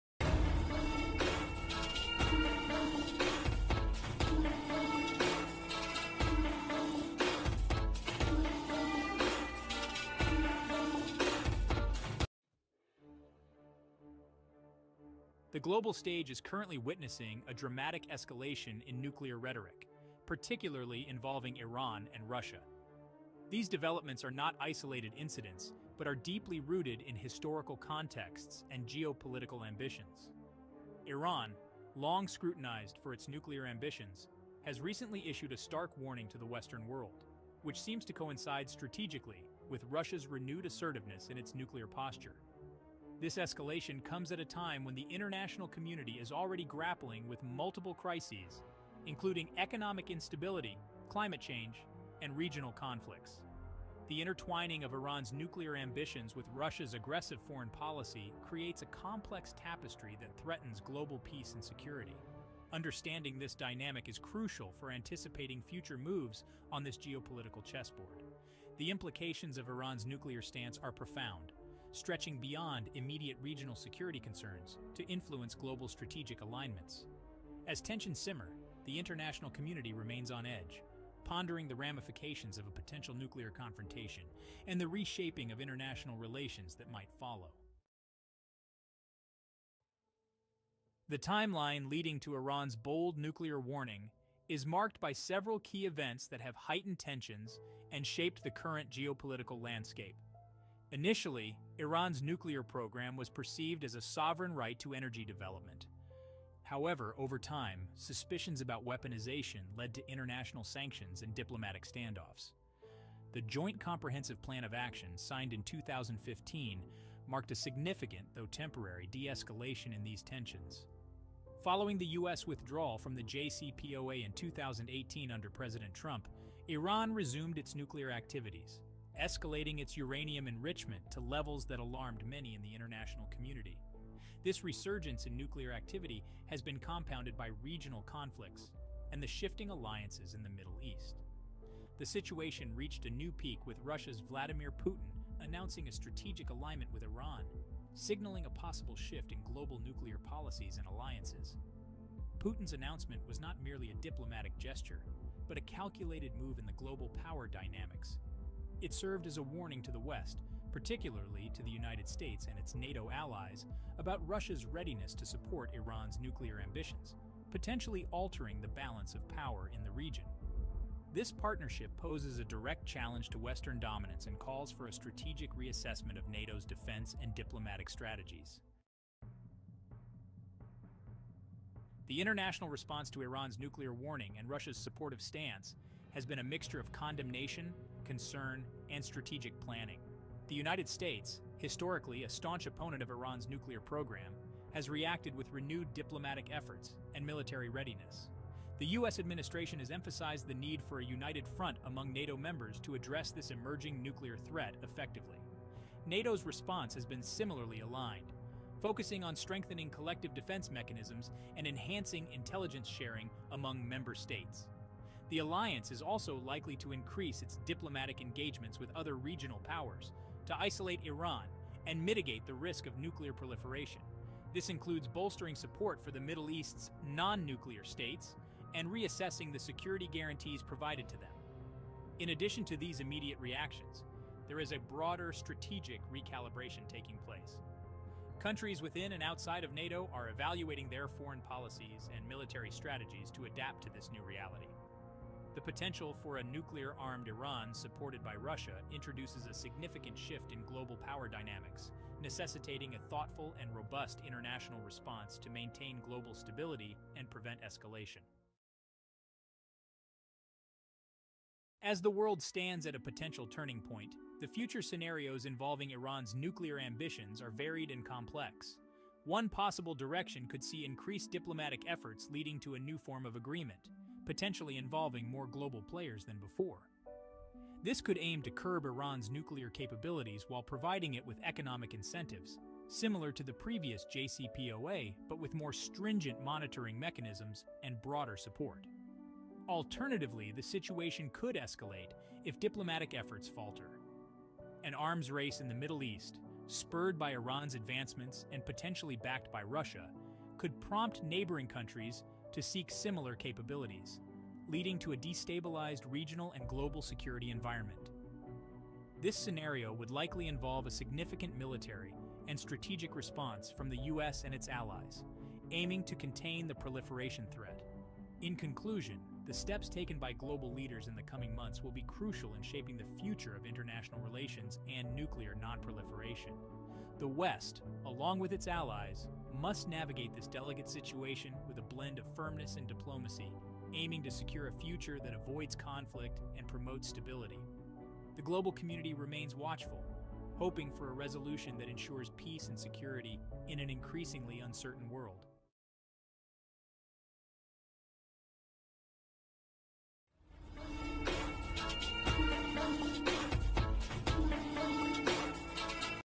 Iran's Bold Nuclear Stand- A Warning to the west |Fact Flow Unveiled
# Iran's Nuclear Warning To West After Putin's Announcement

## 🗒️ Answer
Iran's nuclear chief issued a warning to the U.S.-led West following Putin's announcement, asserting Iran's readiness to confront arrogance [[2] This warning comes amid rising tensions in the geopolitical landscape, potentially signaling Iran's assertiveness regarding its nuclear capabilities.